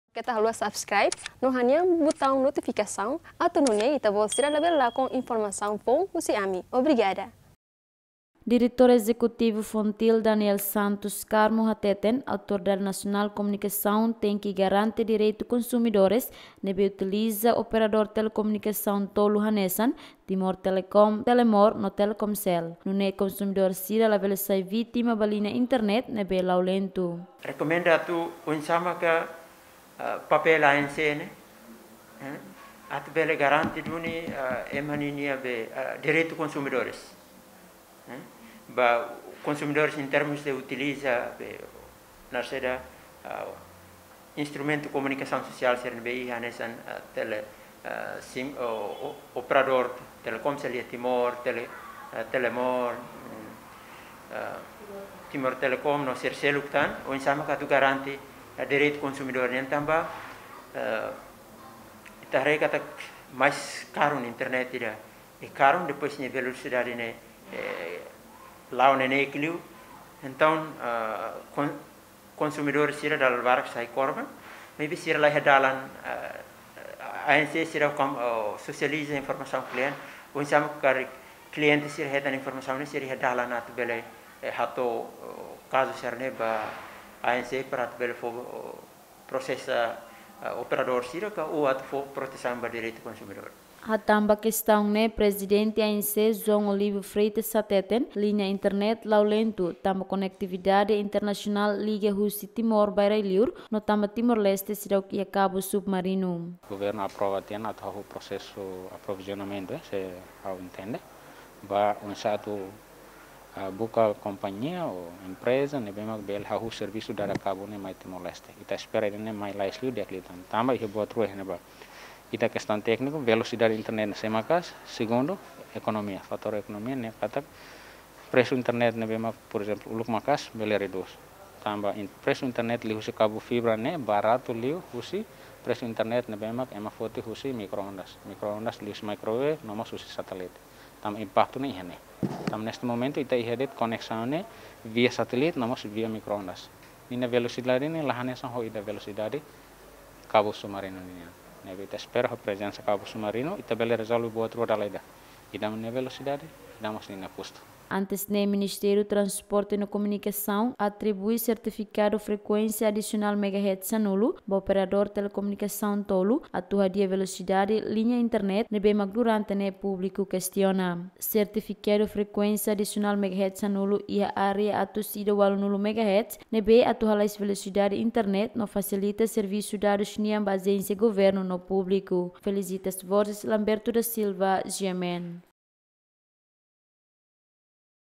FONGTIL husu ANK rezolve problema liña internet
FONGTIL husu Autoridade Nasional Komunikasaun atu rezolve problema liña internet ne’ebé kontinua lao lentu